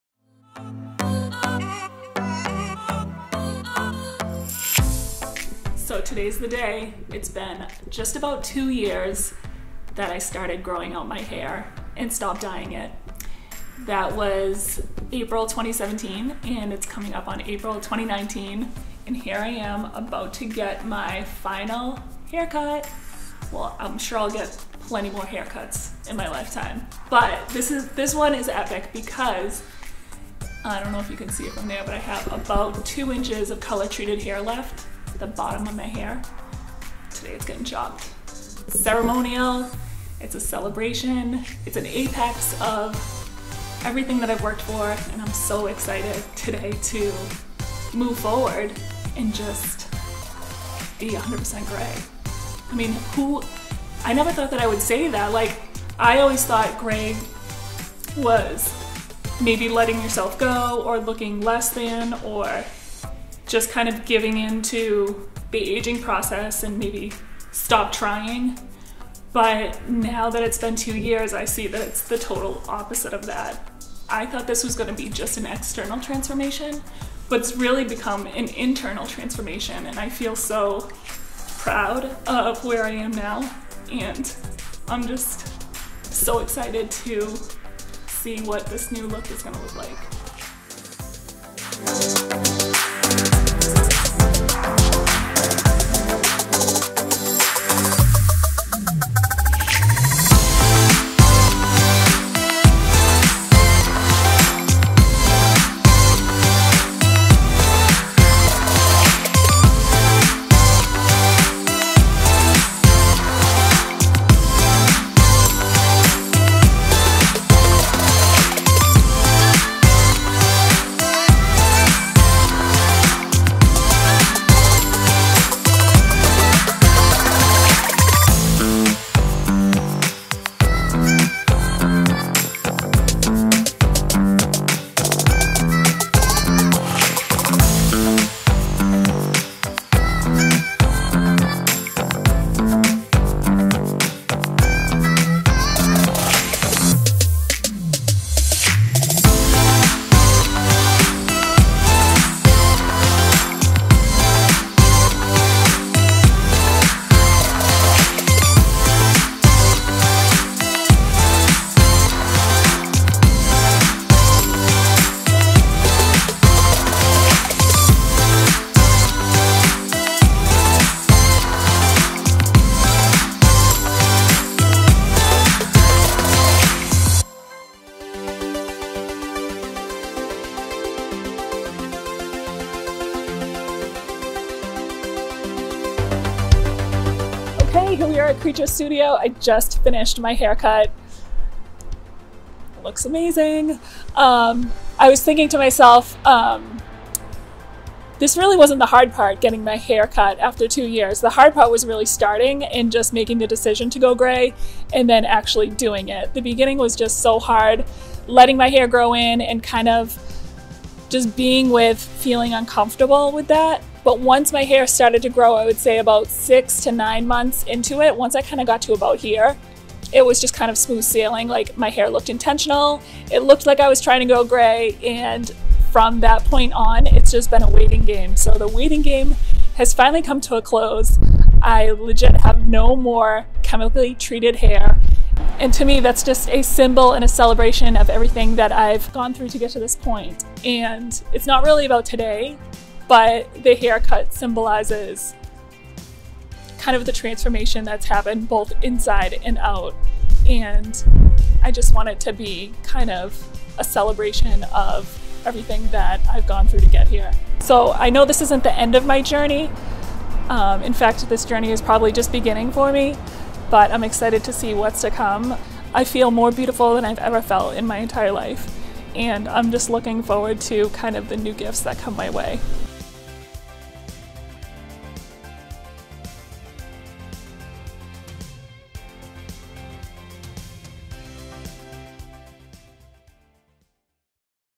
Final Going Gray Haircut
FINAL HAIRCUT!!! It's been exactly 2 years since I stopped dying my hair, and for the first time since I was 19 years old, my hair is 100% natural. HOLLA!

Getting this final cut wasn't the hard part. Committing to the process and sticking to it is really where I had to dig in. I wanted to film and share this cut as a symbol, and celebration, of this transformation. It's one that has changed me, both internally and externally, for the better.